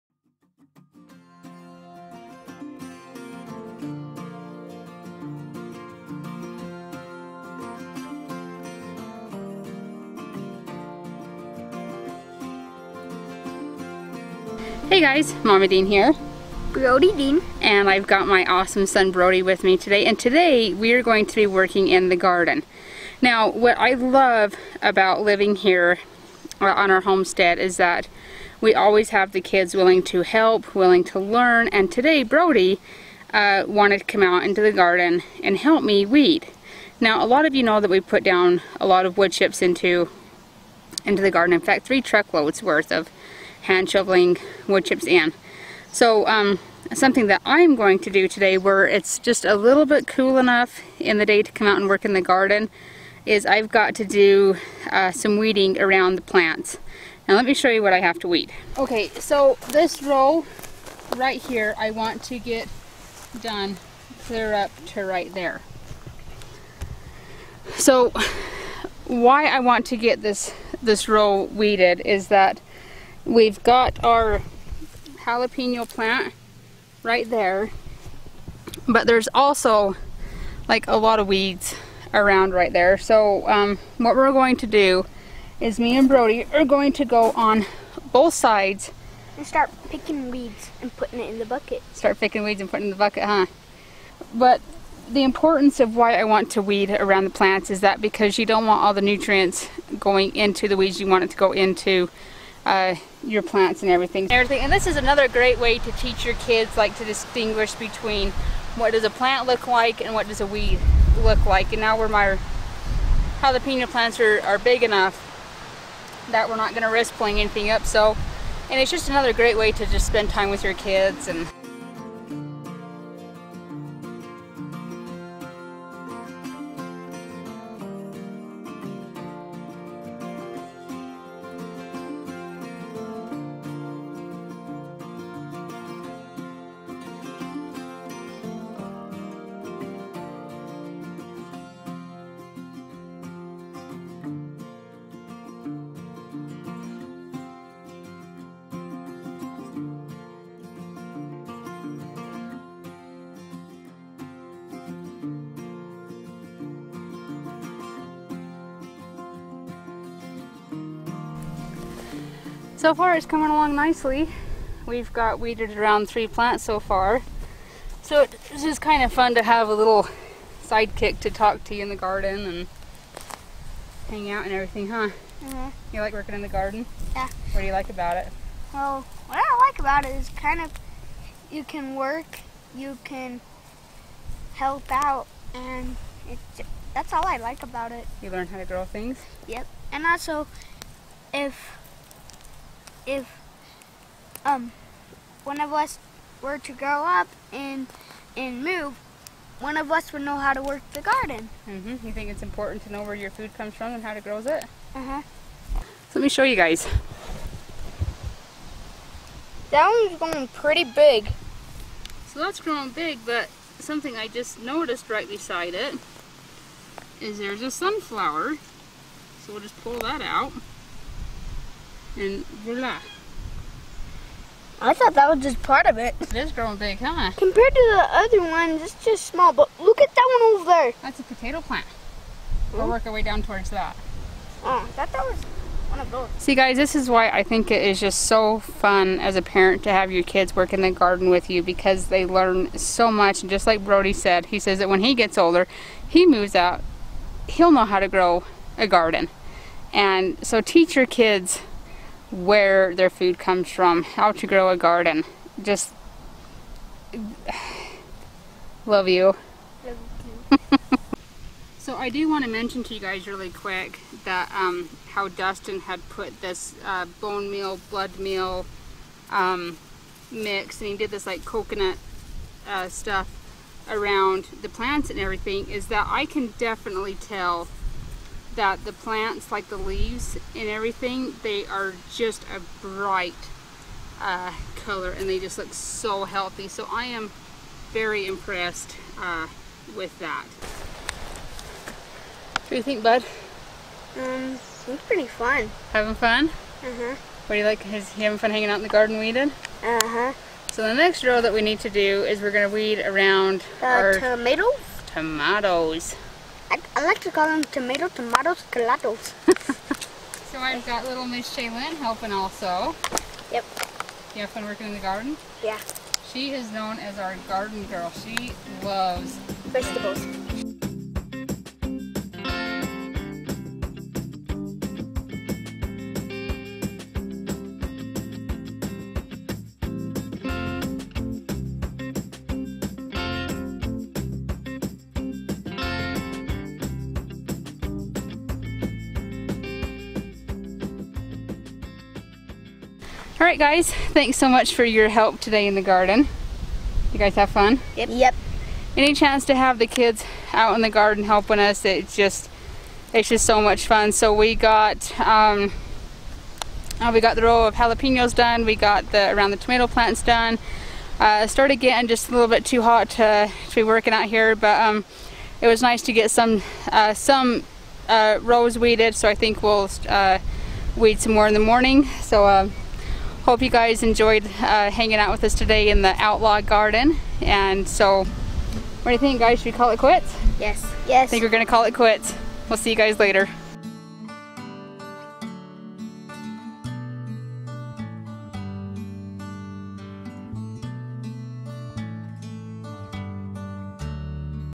MAKING MEMORIES IN THE GARDEN
today i wanted to show the fun and the memories that you can make in the garden with the kids. i had help from brody and shaylynn in the garden. we got weeds pulled and had fun.

SNAIL MAIL
DUSTIN DEAN
ELSINORE UTAH, 84724